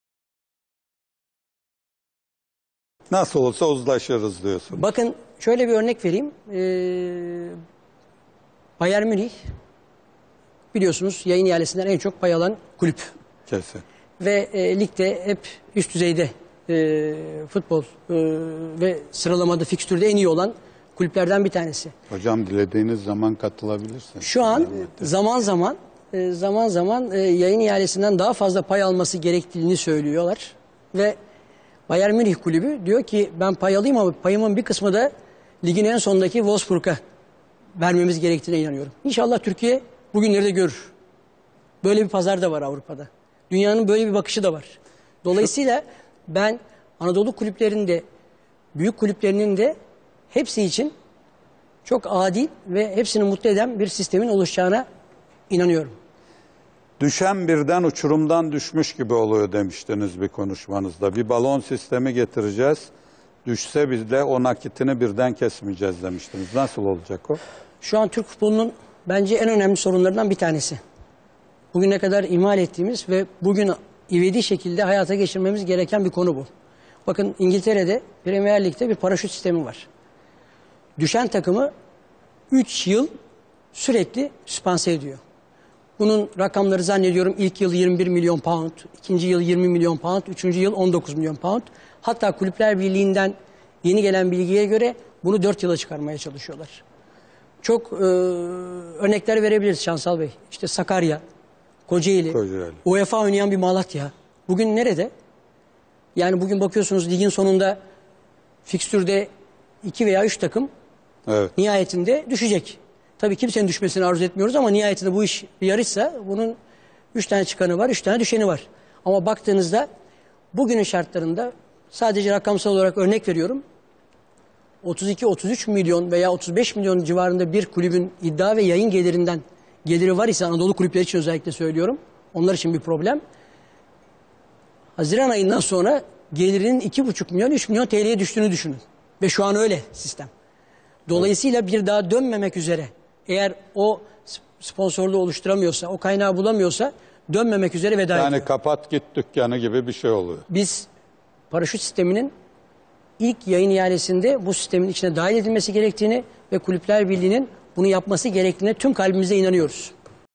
goksel gumusdag parasut odemesi aciklamasi
Göksel Gümüşdağ'ın Paraşüt mdemesi ile ilgili düşündükleri.